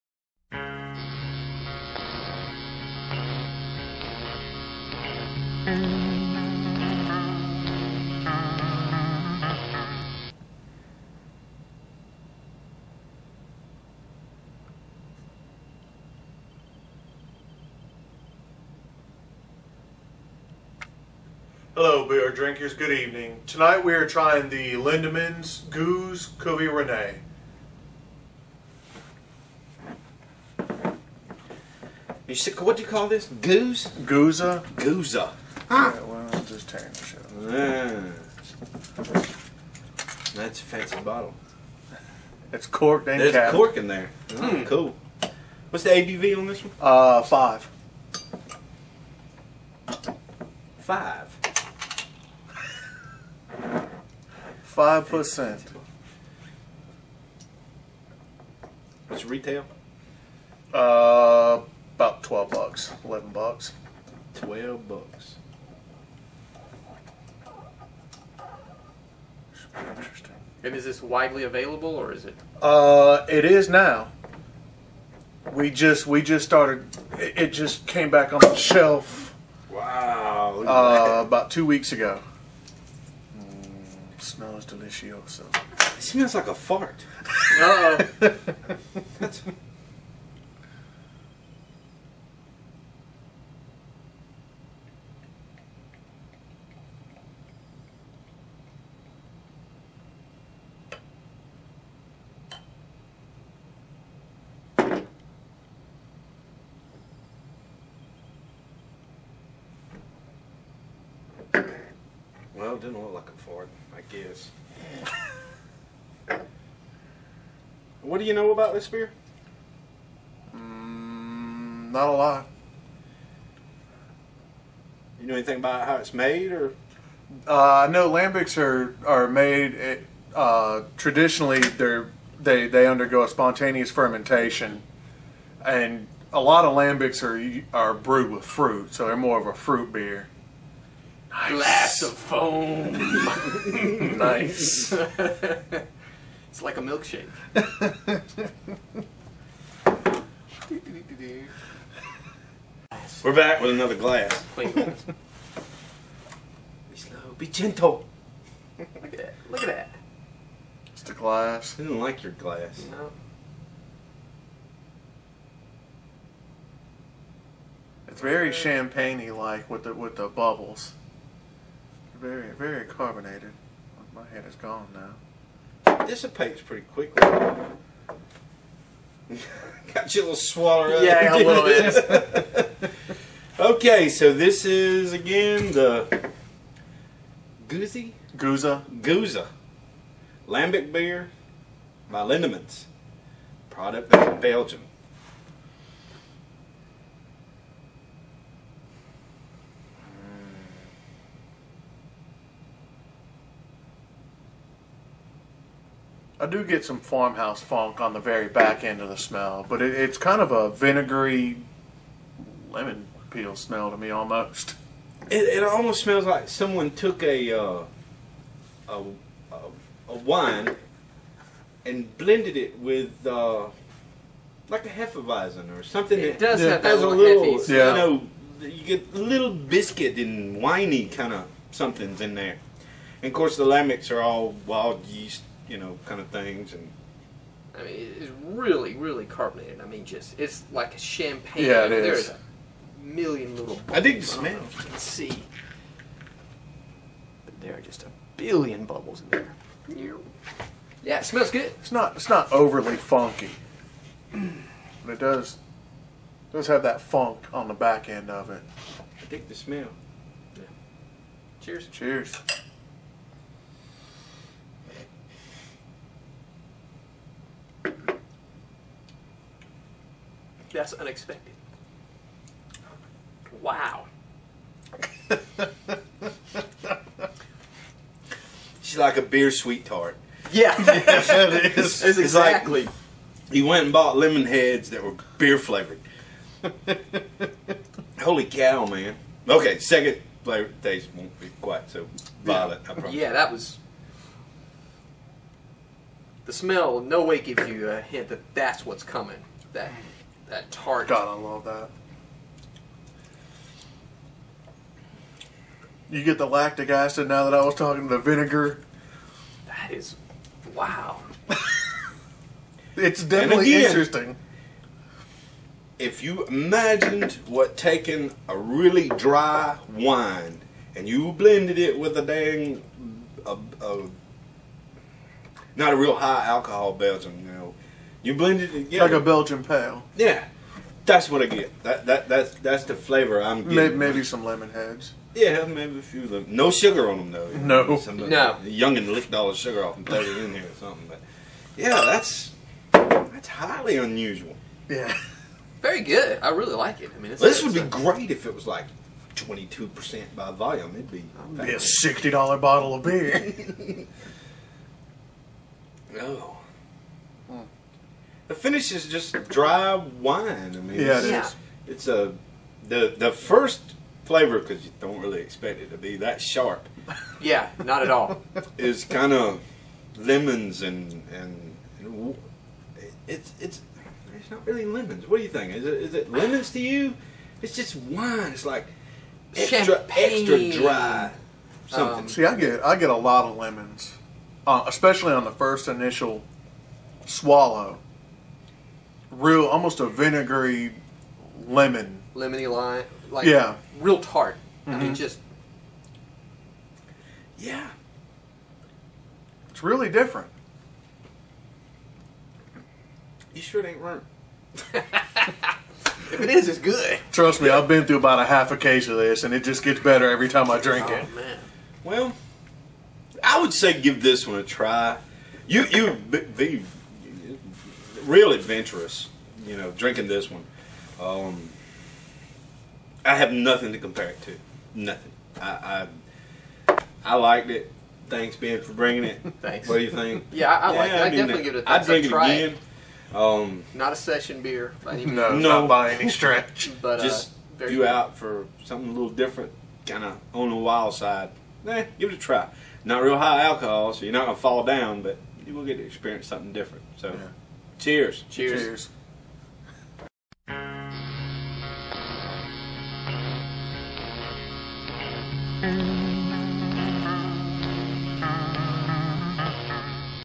beer review: Lindemans Gueuze Cuvee Renee
reviews the Lindemans Gueuze Cuvee Renee. It's an odd mixture of beer and sour candy that is not for the faint of heart. The flavors are so strong that I can only describe it as lemoney violence. It just assaults your taste buds over and over again.